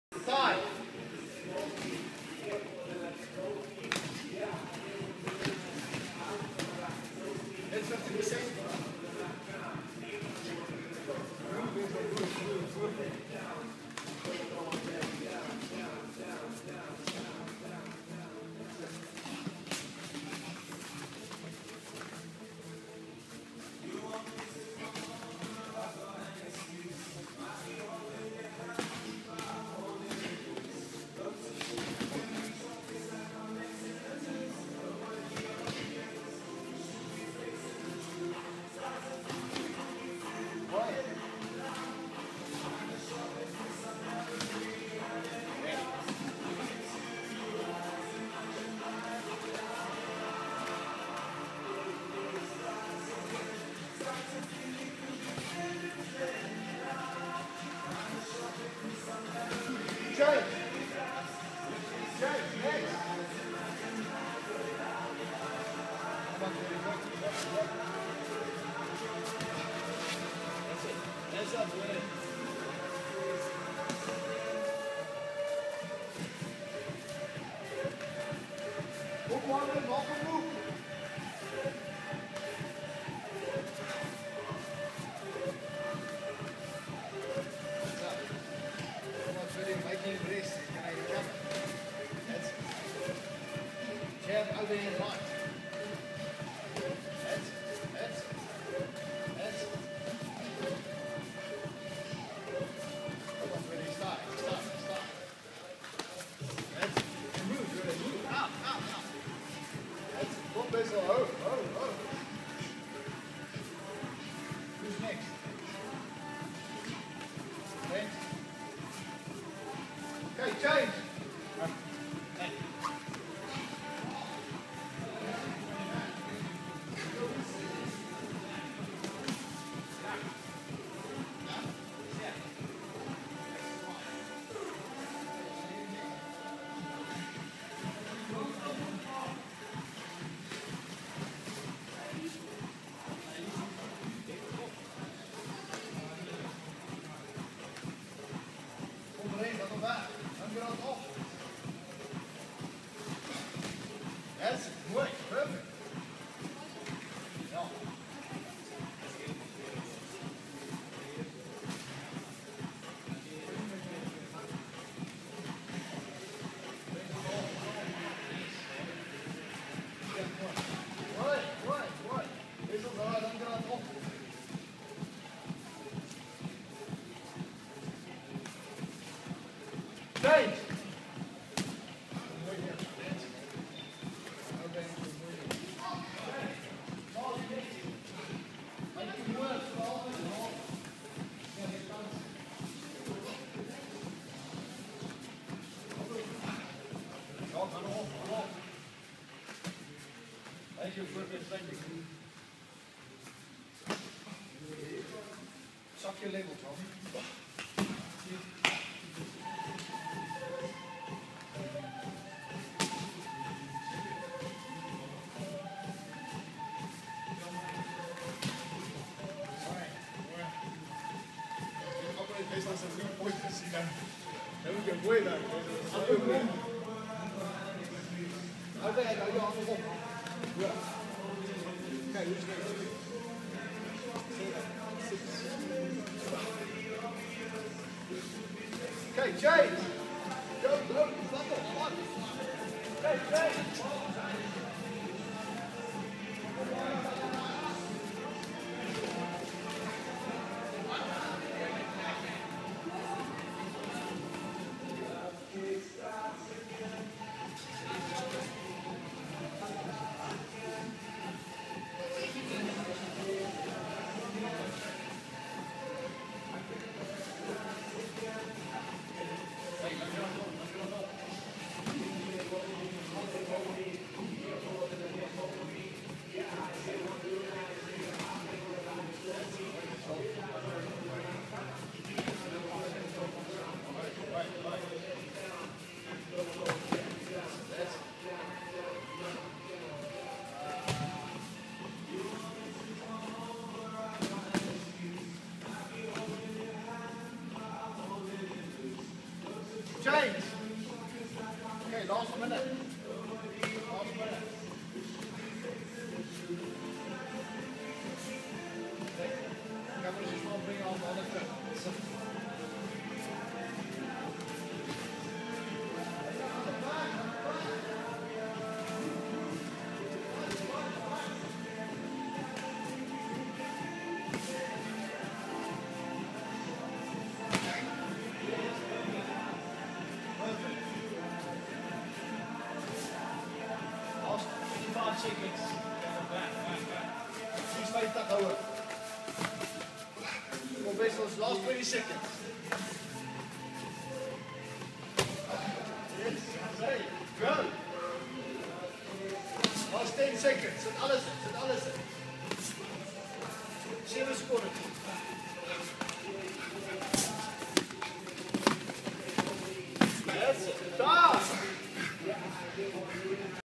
Wessel mostert
Wessel mostert doing a 6 min iron man sparring 1 min a new guy